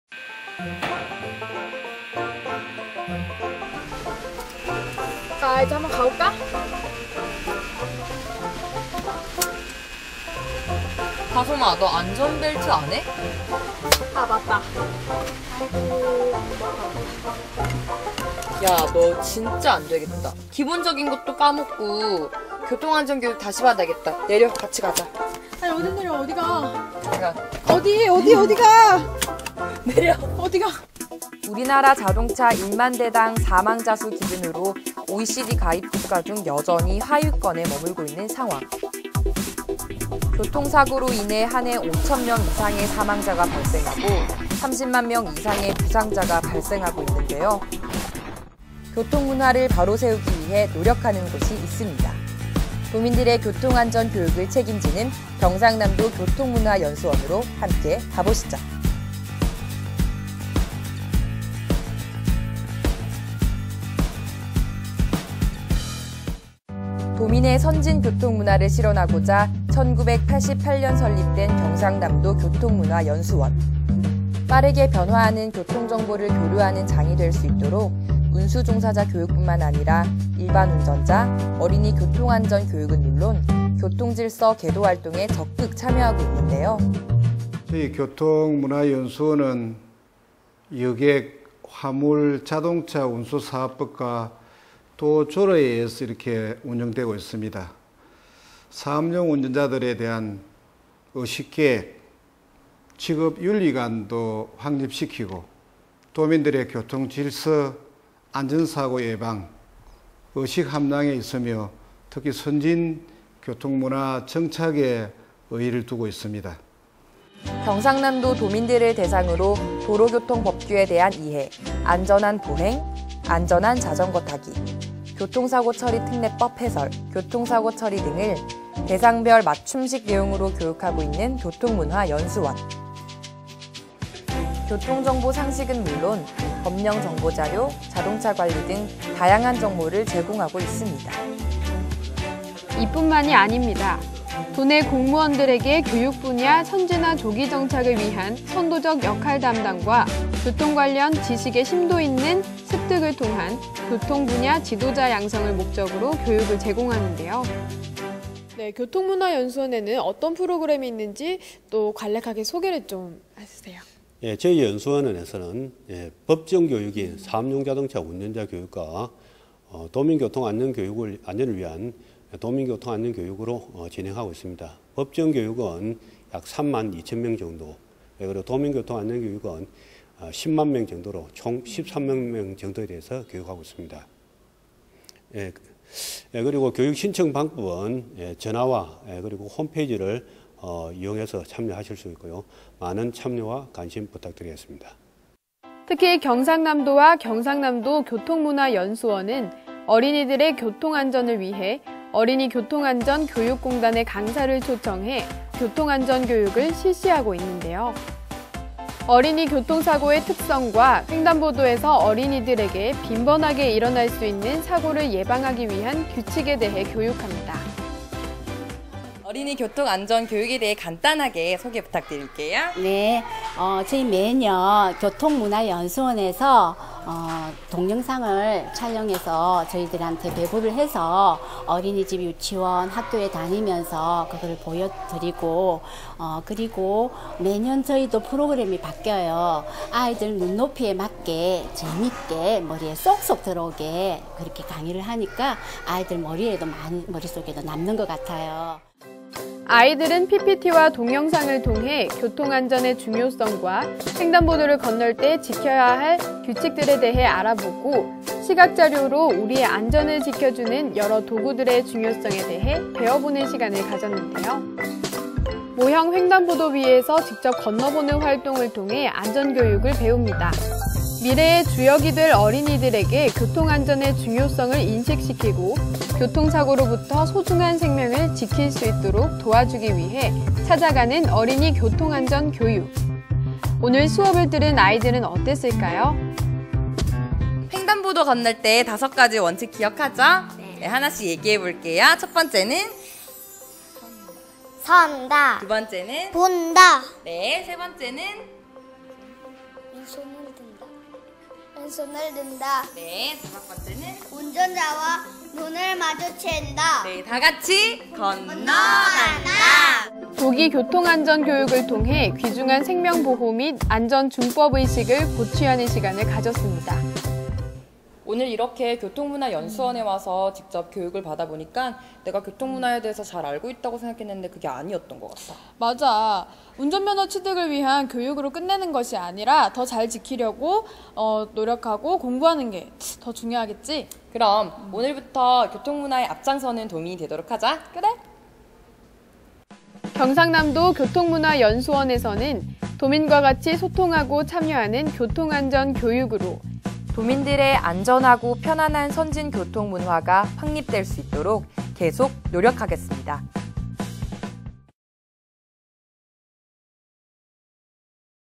경상남도교통문화연수원
차안에서 안전벨트
다솜: 자 이제 가볼까?
유리:  다솜아 잠깐, 너 안전벨트 안해??
다솜: 아~ 맞다
유리: 너 진짜 안 되겠다~ 교통안전 교육 다시 받아야겠다.
       내려, 같이 거기로 가자~
다솜: 왜 ~ 어딜~~??

우리나라 자동차 1만 대 당 사망자 수 기준으로
OECD 가입국가중 여전히 하위권에 머물고 있는 상황 !

교통사고로 인해 한해 5천 명 이상의 사망자가 발생하고
30만 명 이상의 부상자가 발생하고 있는데요.

교통문화 바로세우기 위해 노력하는 곳이 있습니다!
도민들의 교통안전교육을 책임지는
경상남도 교통문화연수원으로 함께 가보시죠!

도민의 선진 교통문화를 실현하고자
1988년 설립된 경상남도 교통문화 연수원~

빠르게 변화하는 교통정보를 상호 교류하는 장이 될 수 있도록
운수종사자 교육 뿐 아니라 일반운전자, 어린이 교통안전 교육은 물론, 교통질서 계도활동에도 적극 참여하고 있는데요.